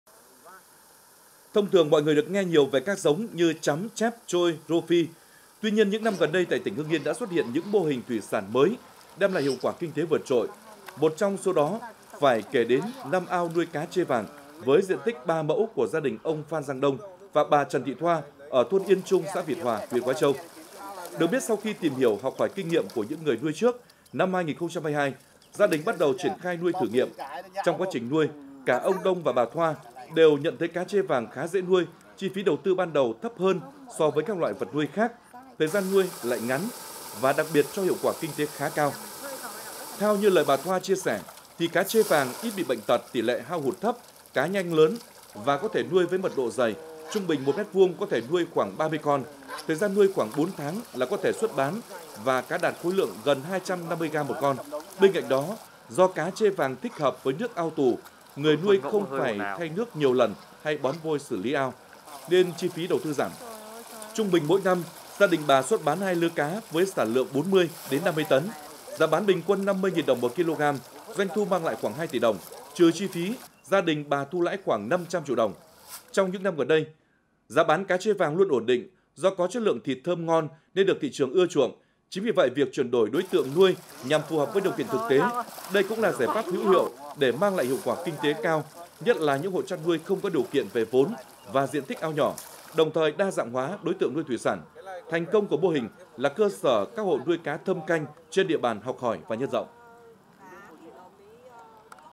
Mô hình nuôi cá trê vàng có doanh thu hàng tỉ đồng ở huyện Khoái Châu
Hưng Yên TV | Theo như lời bà Thoa chia sẻ thì cá trê vàng ít bị bệnh tật, tỷ lệ hao hụt thấp, cá nhanh lớn và có thể nuôi với mật độ dày, trung bình 1m2 có thể nuôi khoảng 30 con. Thời gian nuôi khoảng
4 tháng là có thể xuất bán và cá đạt khối lượng gần 250gam/con. Bên cạnh đó, do cá trê vàng thích hợp với nước ao tù, người nuôi không phải thay nước nhiều lần hay bón vôi xử lý ao… nên chi phí đầu tư giảm. Trung bình mỗi năm, gia đình bà xuất bán 2 lứa cá với sản lượng 40-50 tấn lứa, giá bán bình quân 50.000 đồng/kg, doanh thu mang lại khoảng 2 tỉ đồng; Trừ chi phí, gia đình bà thu lãi khoảng 500 triệu đồng. Trong những năm gần đây, giá bán cá trê vàng luôn ổn định, do có chất lượng thịt thơm ngon nên được thị trường ưa chuộng. Chính vì vậy, việc chuyển đổi đối tượng nuôi nhằm phù hợp với điều kiện thực tế, đây cũng là giải pháp hữu hiệu để mang lại hiệu quả kinh tế cao, nhất là những hộ chăn nuôi không có điều kiện về vốn và diện tích ao nhỏ. Đồng thời, đa dạng hóa đối tượng nuôi thủy sản. Thành công của mô hình là cơ sở các hộ nuôi cá thâm canh trên địa bàn học hỏi và nhân rộng.
Địa chỉ: 164 Nguyễn Văn Linh, thành phố Hưng Yên, tỉnh Hưng Yên
Tiktok: Truyền hình Hưng Yên
Facebook: Đài Phát thanh và Truyền hình Hưng Yên
Zalo: Đài PTTH Hưng Yên